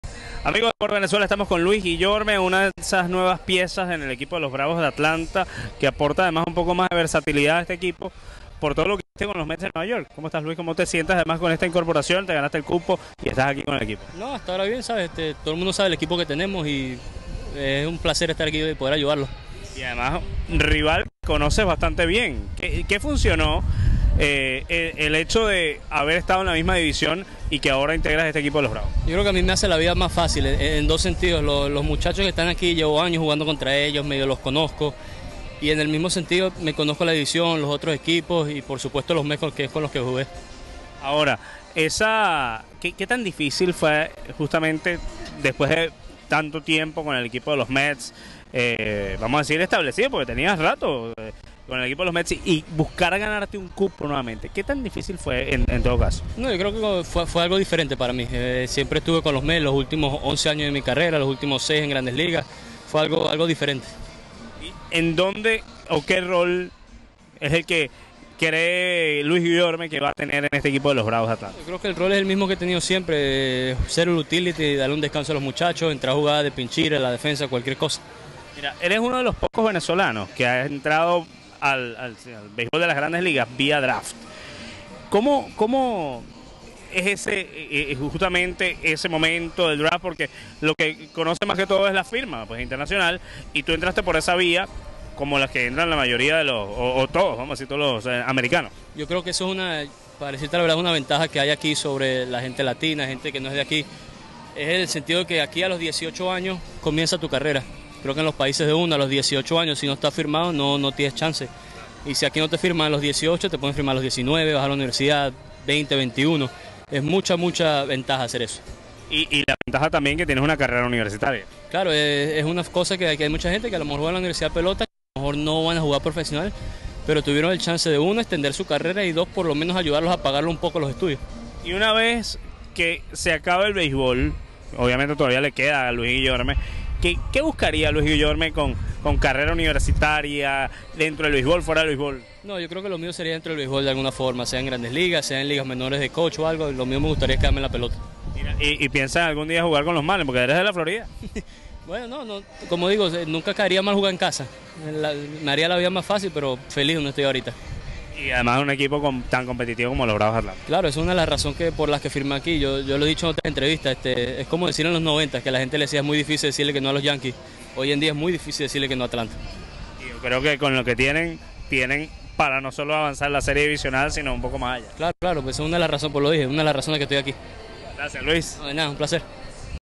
Entrevista con Luis Guillorme, utility de los Bravos de Atlanta
Conversamos con Luis Guillorme⚾️🇻🇪, utility de los Bravos de Atlanta, en el loanDepot park🏝️